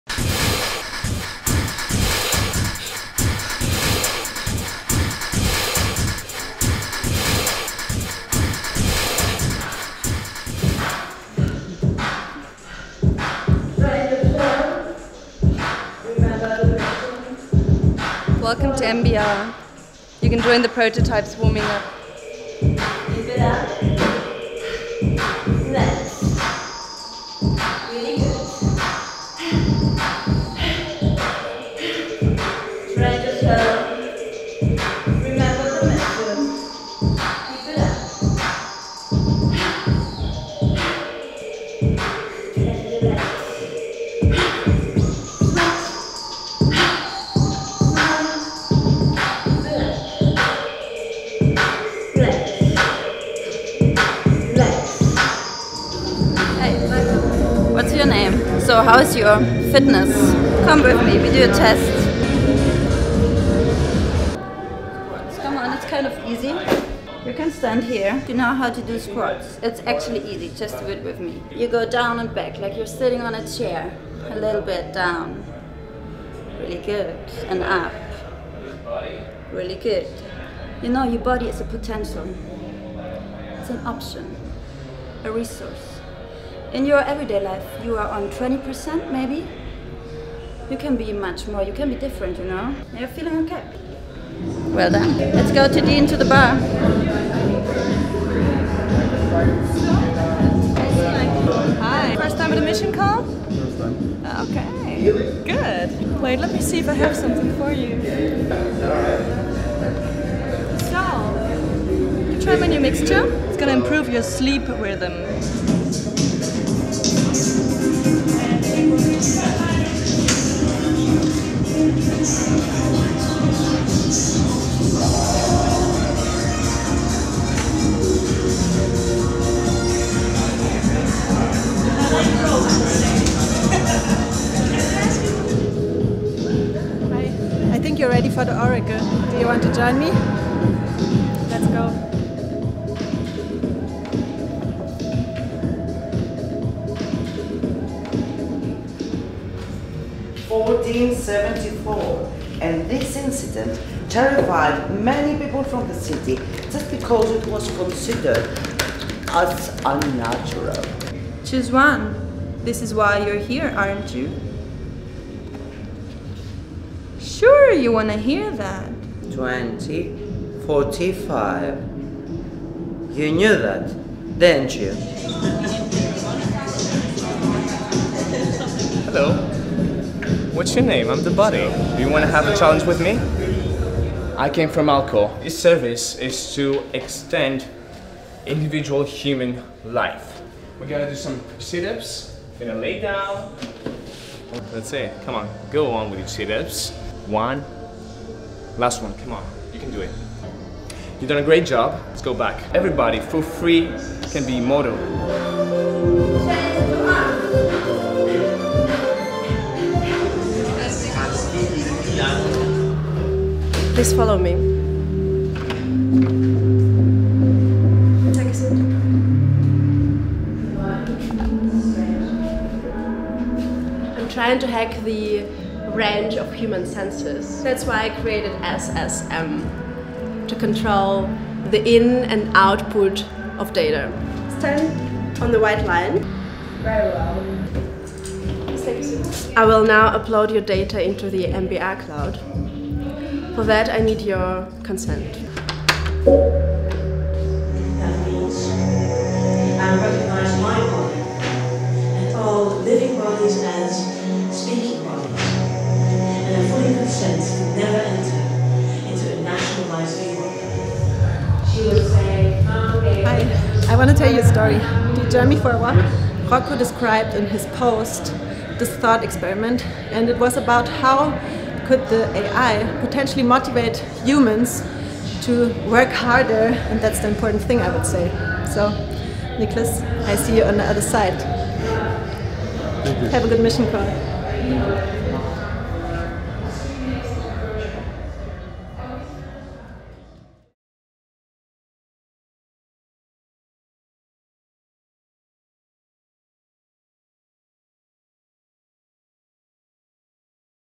Medusa Bionic Rise (2018) by The Agency | Athens Biennale 2018
Medusa Bionic Rise is a complex performance-installation, a blast of actions explodes before us. This is the dawn of an era in which wellness transforms into the cult of healthism. Medusa Bionic Rise, through its numerous layers, can be perceived as a visual walkthrough to post-humanism.

Installation, performance, video
Courtesy of the artists
With the kind support of the Institut für Auslandsbeziehungen, Goethe-Institut Athen, and the City of Munich
Video credits:
Curated by Christos Dimolikas
Directed by Nicolas Pottakis
Shot by Giorgos Alexiadis, Christos Dimolikas, Avrilios Karakostas, Stelios Moraitidis, Nicolas Pottakis
Edited by Giorgos Tsapis
Production by JNL+
Production Design by Areti Vardaki